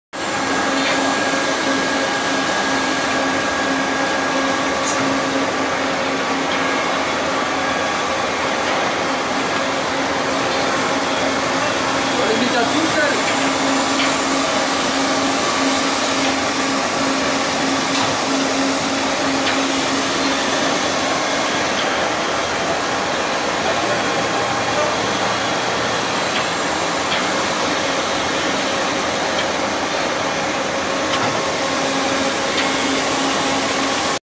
Index MS42C CNC Multi Spindle Lathe
Pick up attachment,
Drilling attachments,
All axes CNC controlled,
Fully equipped,
Pietro Cucchi magazine bar loader,